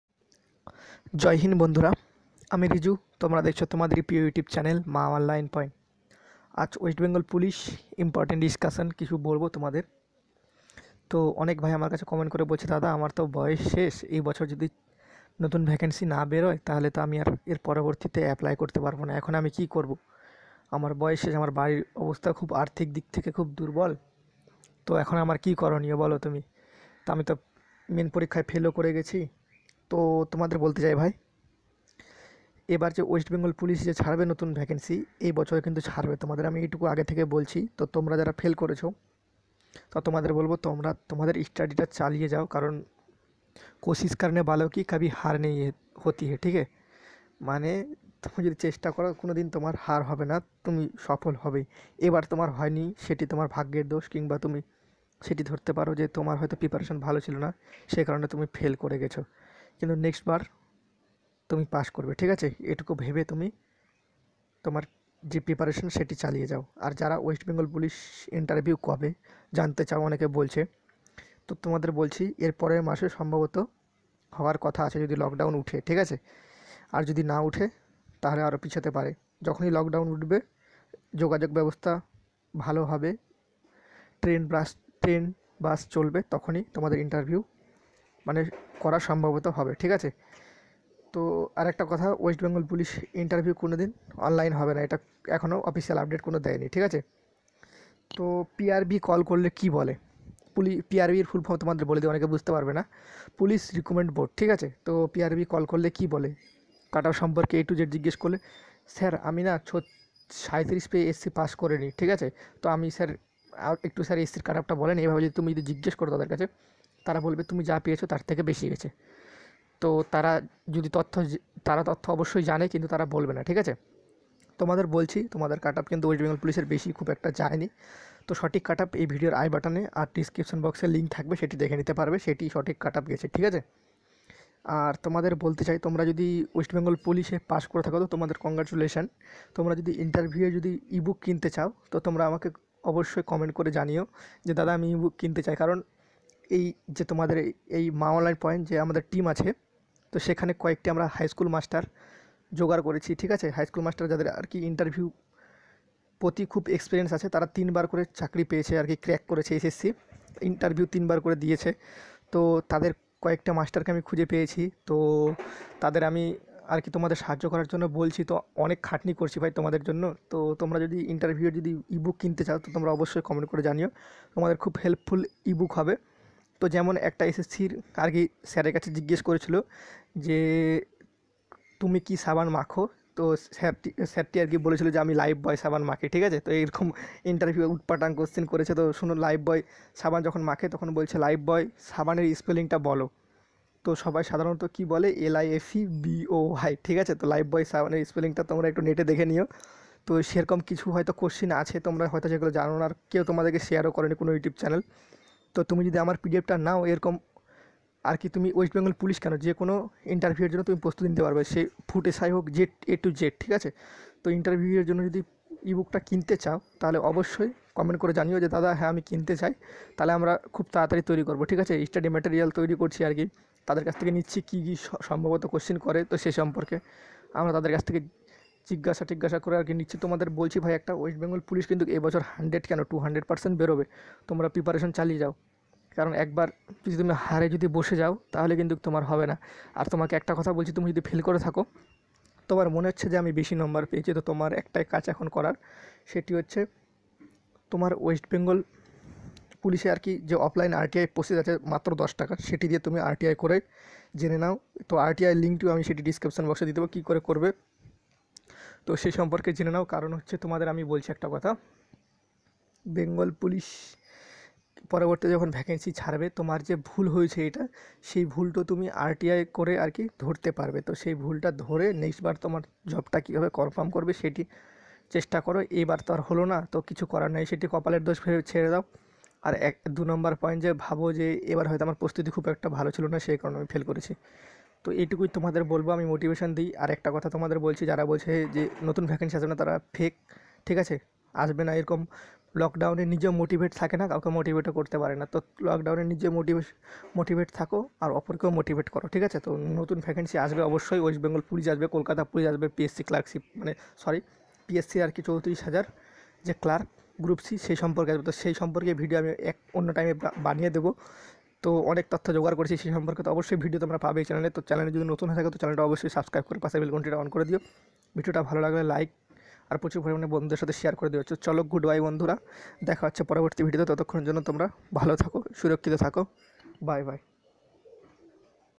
WBP Important Discussion/WBPRB PRB কল করলে কি বলে/Interview/New Vacancy 2020
WBP Main যারা ফেল করেছে তারা কি করে নিজের নম্বর দেখবে/কাটো-অফ বেশি গেলো কেন  সঠিক Cut-Off কত  গেলো